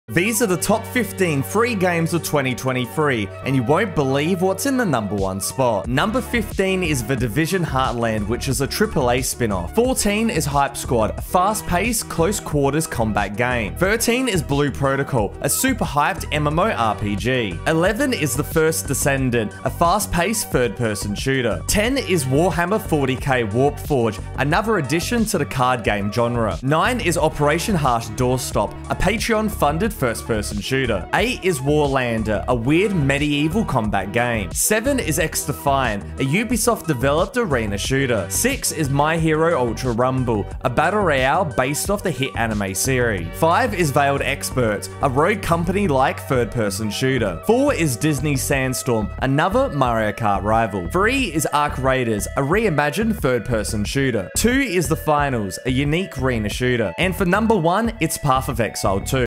The top 15 free games of 2023!
These are the best free to play games on Steam in 2023!